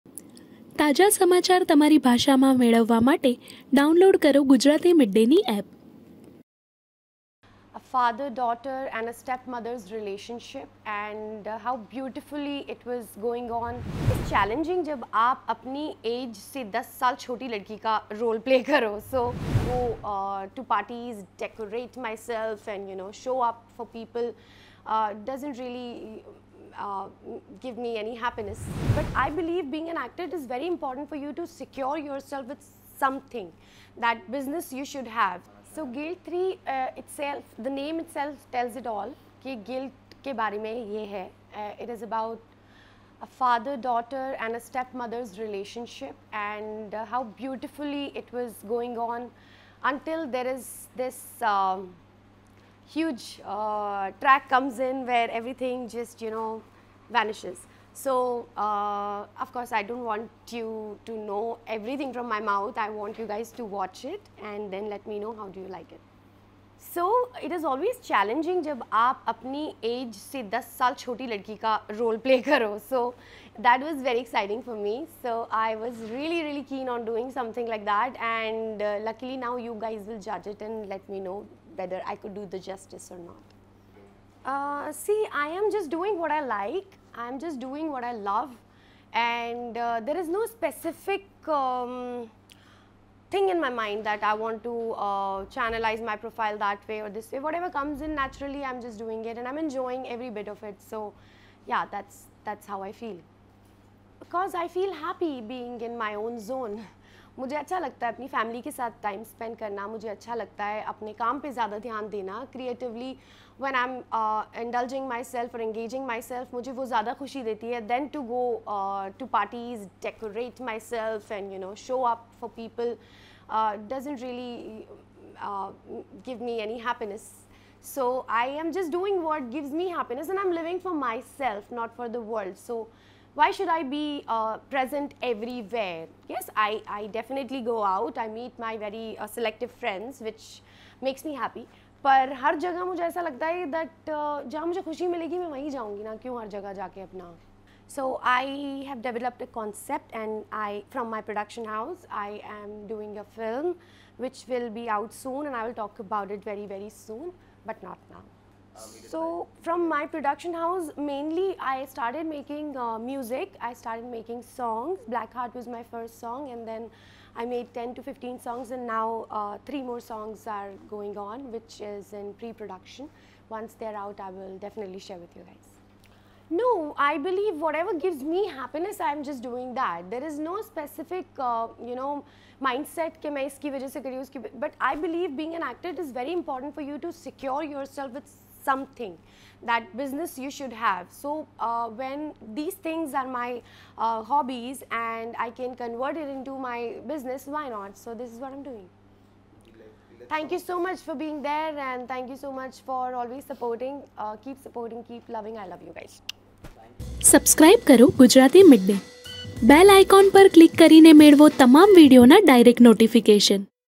હું મારે માટે જીવું છું, દુનિયા માટે નહીં
'સપના બાબુલ કા... બિદાઈ'ની એક્ટ્રેસ સારા ખાને અપકમિંગ ફિલ્મ 'ગિલ્ટ 3' વિશે કરી ચર્ચા

'સપના બાબુલ કા... બિદાઈ'માં તેના રોલ માટે જાણીતી ટીવી સ્ટાર સારા ખાન તેની લેટેસ્ટ ફિલ્મ 'ગિલ્ટ 3'ની તૈયારી કરી રહી છે. તેણે પોતાની એક્ટિંગ જર્ની વિશે કેટલીક વાતો શૅર કરી, પોતાના કરતા દસ વર્ષ નાના પાત્રનું ચિત્રણ કરતી વખતે તેને જે પડકારોનો સામનો કરવો પડ્યો તેની ચર્ચા કરી. અભિનેત્રીએ મૂવીની સ્ટોરી વિશે વિગતો પણ જાહેર કરી હતી, જેમાં પિતા, પુત્રી અને સાવકી માતા વચ્ચેની જટિલ ગતિશીલતા પર ધ્યાન કેન્દ્રિત કરવામાં આવે છે. આ આગામી ફિલ્મમાં સારા ખાનની ભૂમિકા અને તેની સફર વિશે વધુ જાણવા માટે આખો વિડિયો જુઓ.